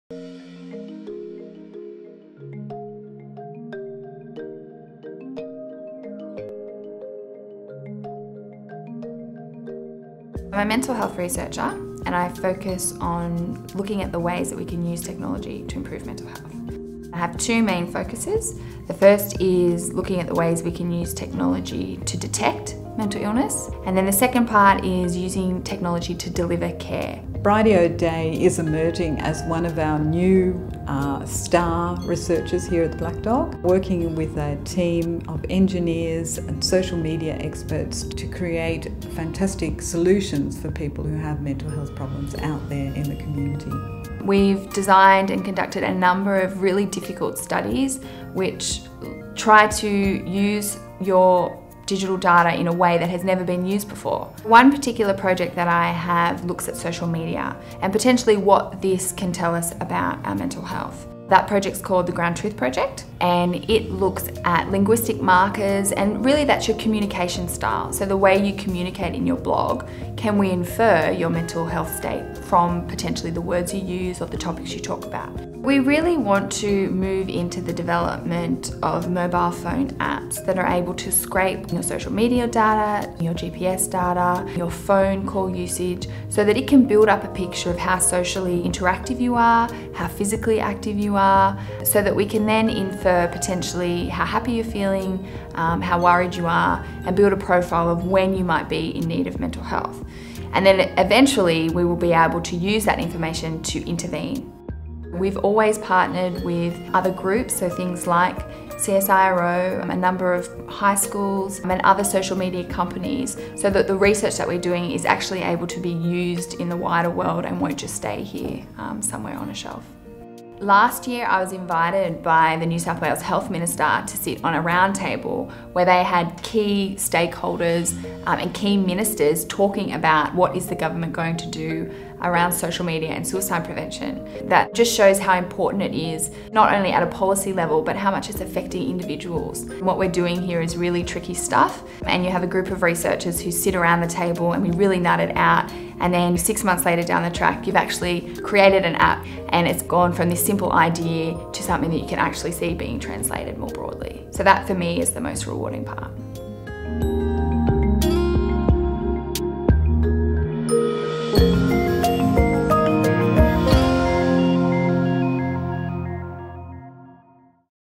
Leading technology for good mental health - Bridianne O'Dea
Bridianne O'Dea is at the forefront of research into how technology can harness the power of social media to detect mental illness and deliver appropriate health care.